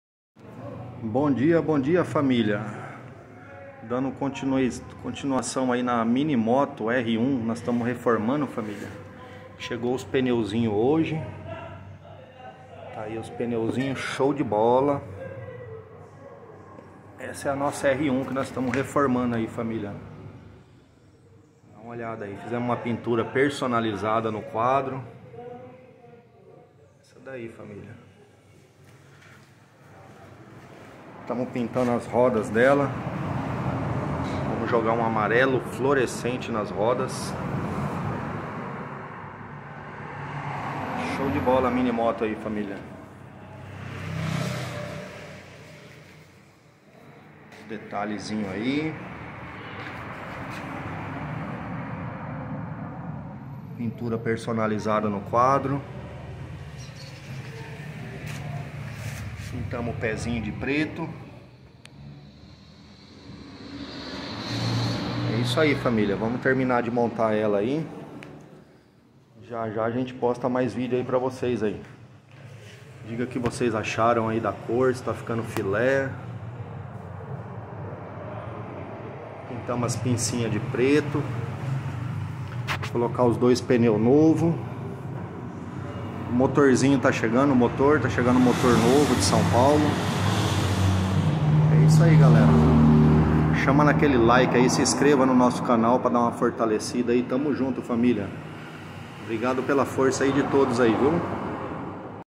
R1 Mais Linda Do Brasil FML 😍😁🙏 Mini Moto Personalizada 🤩🤩
Mini moto R1 Ninja Motor 49 Cilindradas 2 Tempos Reforma da Mini Moto R1 Infantil Gasolina